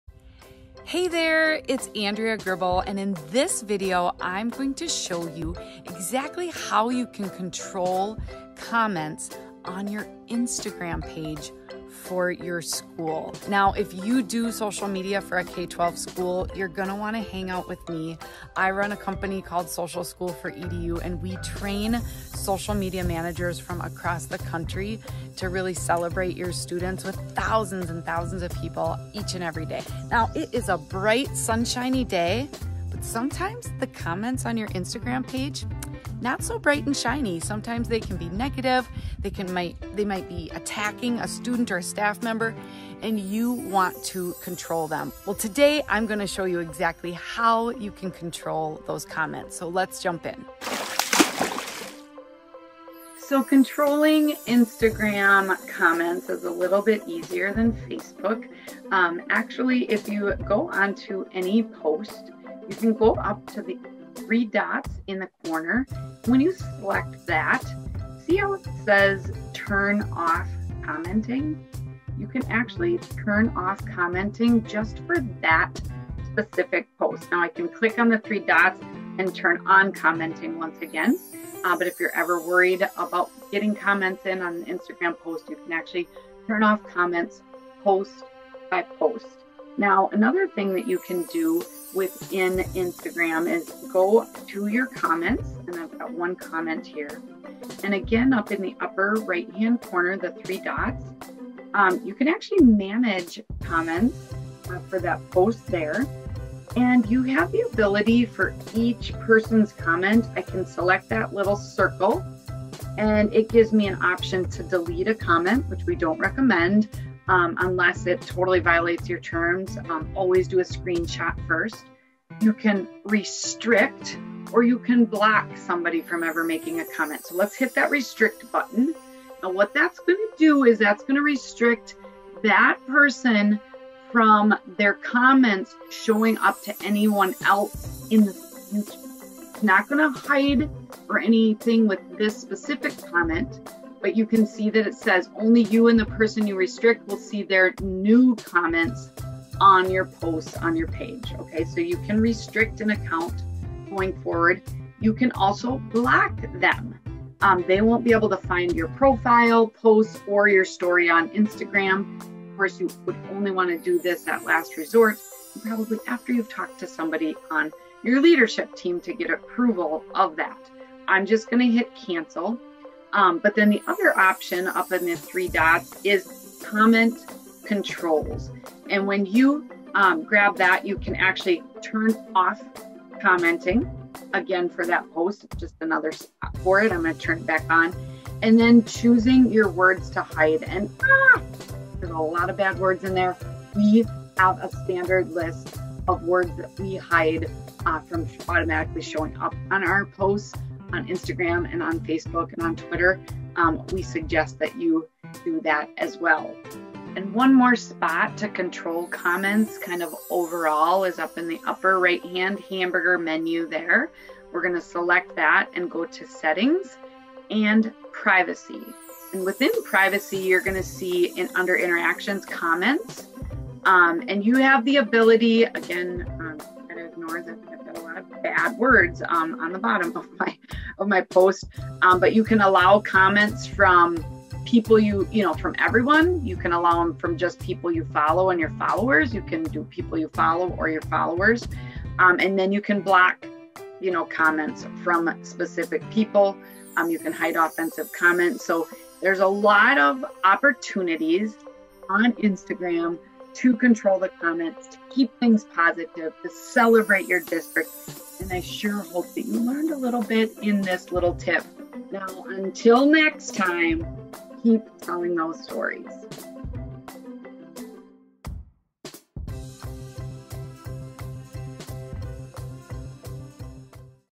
How to Control Comments on Instagram
Hey there, it's Andrea Gribble. And in this video, I'm going to show you exactly how you can control comments on your Instagram page for your school. Now, if you do social media for a K-12 school, you're gonna wanna hang out with me.

I run a company called SocialSchool4EDU, and we train social media managers from across the country to really celebrate your students with thousands and thousands of people each and every day. Now it is a bright, sunshiny day, but sometimes the comments on your Instagram page, not so bright and shiny. Sometimes they can be negative. They might be attacking a student or a staff member and you want to control them.

Well today, I'm gonna show you exactly how you can control those comments. So let's jump in. So controlling Instagram comments is a little bit easier than Facebook. Actually, if you go onto any post, you can go up to the three dots in the corner. When you select that, see how it says, turn off commenting. You can actually turn off commenting just for that specific post. Now I can click on the three dots and turn on commenting once again.

But if you're ever worried about getting comments in on Instagram post, you can actually turn off comments post by post. Now, another thing that you can do within Instagram is go to your comments. And I've got one comment here, and again, up in the upper right-hand corner, that three dots, you can actually manage comments for that post there. And you have the ability for each person's comment. I can select that little circle and it gives me an option to delete a comment, which we don't recommend, unless it totally violates your terms.

Always do a screenshot first. You can restrict, or you can block somebody from ever making a comment. And what that's gonna do is that's gonna restrict that person from their comments, showing up to anyone else. It's not gonna hide or anything with this specific comment.

But you can see that it says only you and the person you restrict will see their new comments on your posts on your page, okay? So you can restrict an account going forward. You can also block them. They won't be able to find your profile, posts or your story on Instagram.

Course you only wanna do this at last resort. After you've talked to somebody on your leadership team to get approval of that, I'm just gonna hit cancel. But then the other option up in the three dots is comment controls and you grab that, you can actually turn off commenting again for that post. Just another for it. I'm gonna turn it back on and then choosing your words to hide.

And there's a lot of bad words in there. We have a standard list of words that we hide from automatically showing up on our posts on Instagram and on Facebook and on Twitter, we suggest that you do that as well. And one more spot to control comments kind of overall is up in the upper right hand hamburger menu there. We're gonna select that and go to settings and privacy.

And within privacy, you're gonna see and under interactions comments, and you have the ability, again, I'm gonna ignore a lot of bad words on the bottom of my posts, but you can allow comments from people, you know, from everyone, you can allow them from just people you follow on your followers.

You can do people you follow or your followers, and then you can block, you know, comments from specific people. You can hide offensive comments. So there's a lot of opportunities on Instagram to control the comments, to keep things positive, to celebrate your district.

And I sure hope see you learned a little bit in this little tip. Now until next time, keep telling those stories.

MORE RESOURCES:
Free Video Training: Learn the simple secrets behind social media for K12 schools!